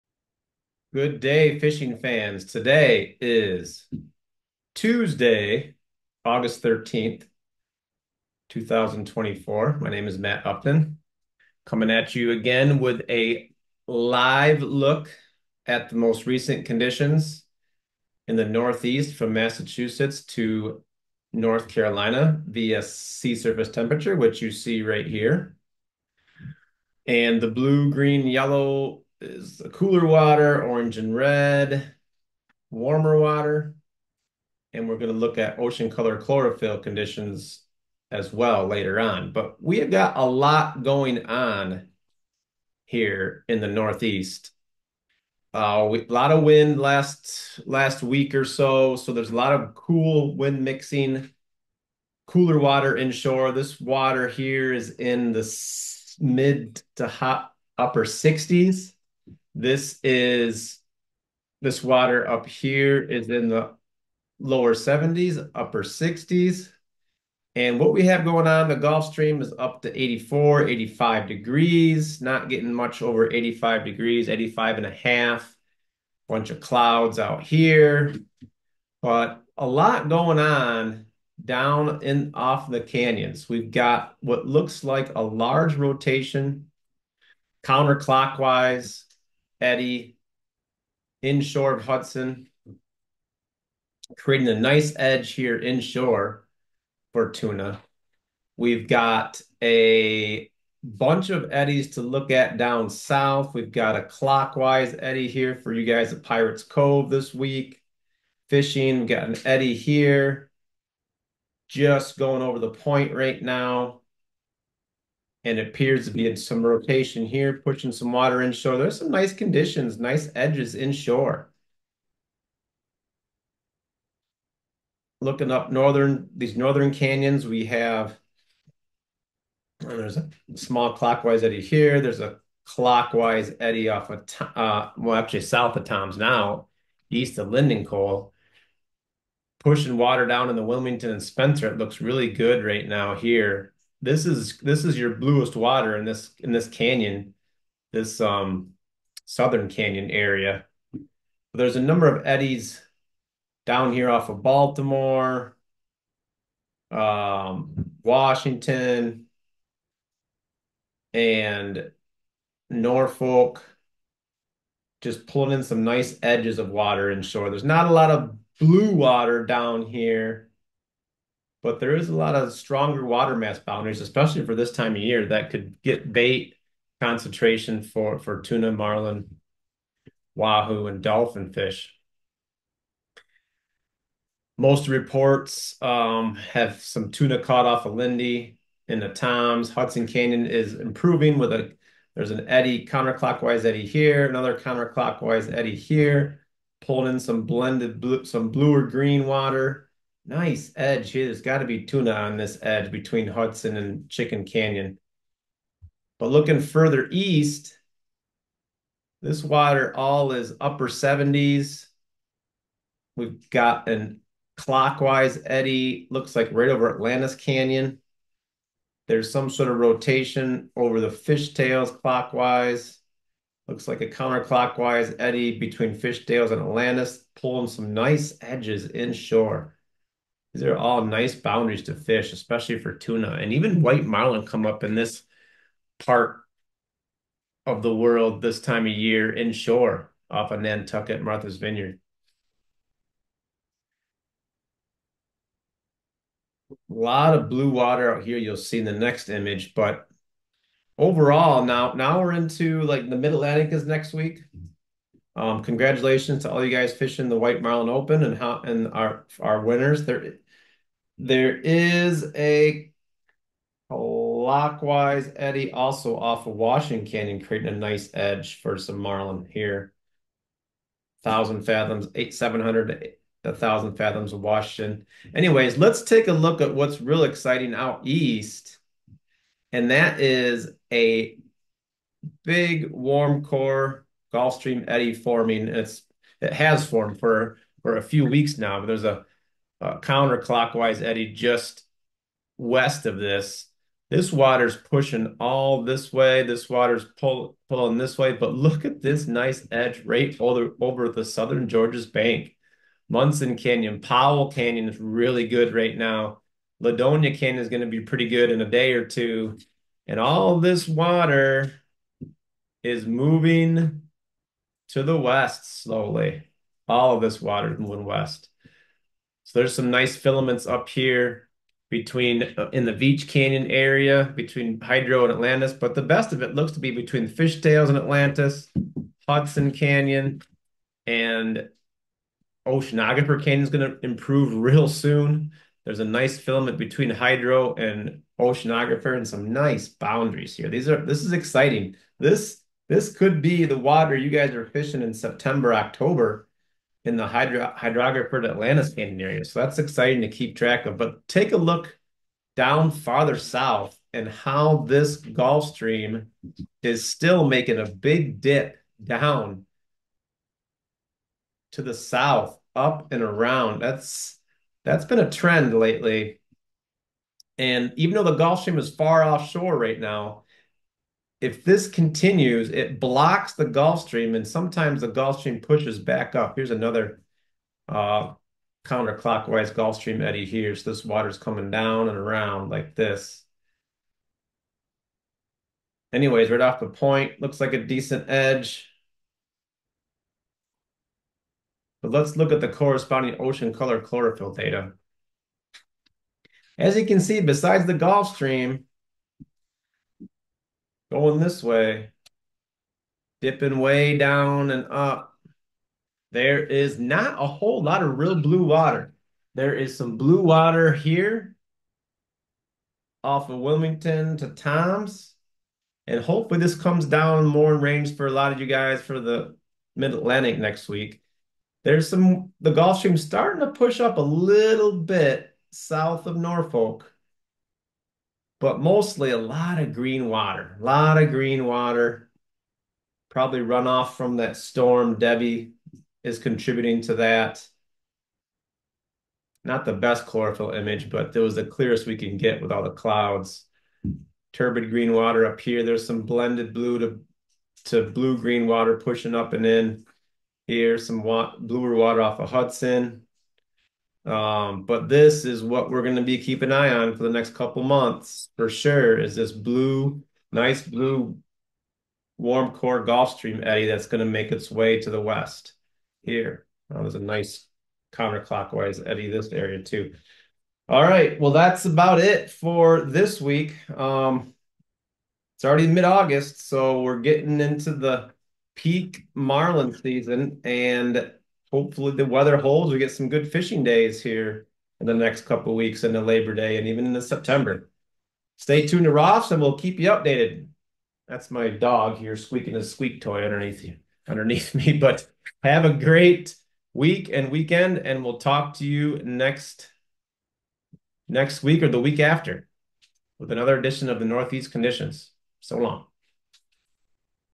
Northeastern U.S. Ocean Conditions Update - 08/13/2024
ROFFS™ brief overview of the overall ocean conditions for the Northeastern U.S. the week of August 13, 2024.